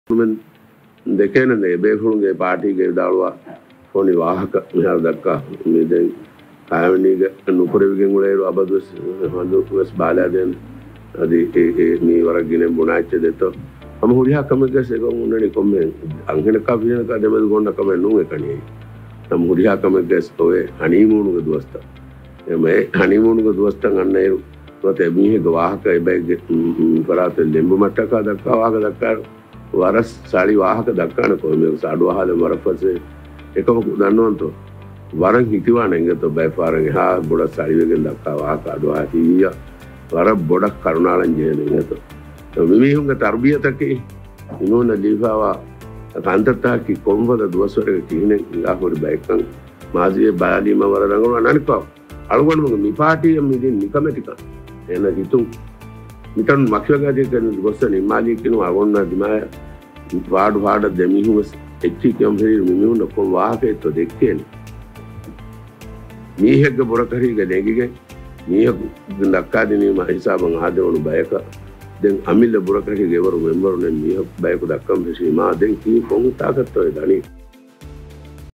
Aa siyaaasee party ufehdhumah Nazim aa Riyaz kuravvaa masahkathah, Qasim
Aa siyaaasee party ufehdhumah Nazim aa Riyaz kuravvaa masahkathah, Qasim: Furathama kolhu foni vaahaka dhekki namaves, fahunlaanee hithi raha!